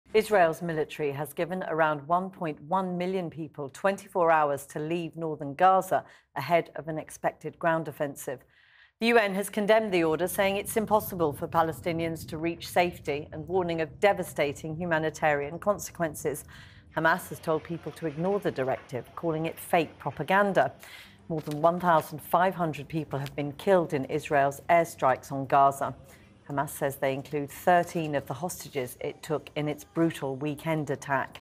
Israel warns Palestinians to leave north Gaza
Israel's military has given around 1.1 million people 24 hours to leave northern Gaza ahead of an expected ground offensive.